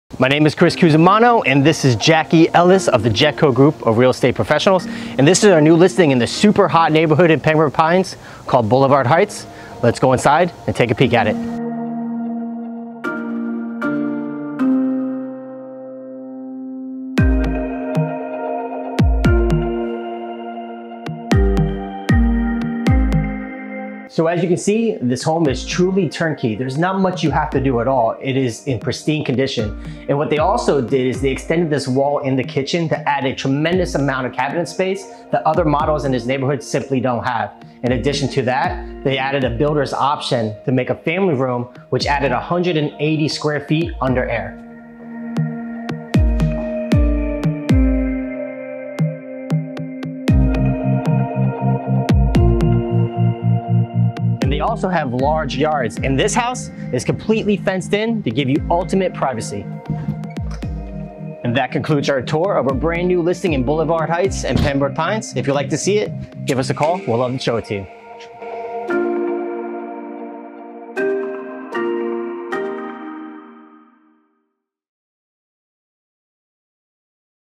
8650 NW 16th St Pembroke Pines, FL Walkthrough Video
Great home located in a quiet residential area of Pembroke Pines. The kitchen has been updated with great tiling on the floors and lots of counter top space that makes prepping your meals easy! The home has hurricane shutters throughout and a large open backyard that is great for entertaining. What made the current owners fall in love with this home and area is how family-oriented it is, with several community events being hosted throughout the year like barbecues and more!